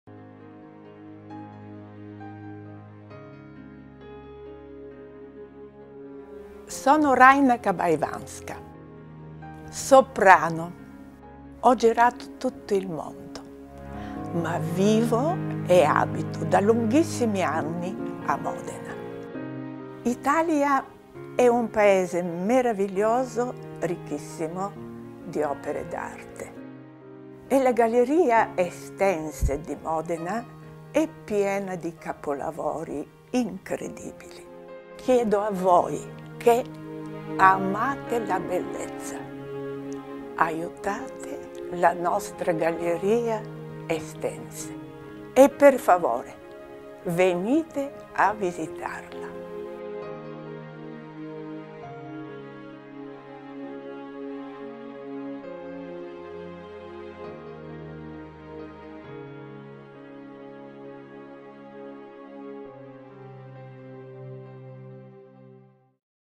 Raina Kabaivanska per For Italy: aiutate la Galleria Estense di Modena
Raina Kabaivanska, soprano, sostiene la campagna di crowdfunding promossa da For Italy per la realizzazione di un innovativo piedistallo antisismico che consentirà di esporre in totale sicurezza il busto di Francesco I d'Este, capolavoro di Gian Lorenzo Bernini, orgoglio e simbolo della Galleria Estense di Modena.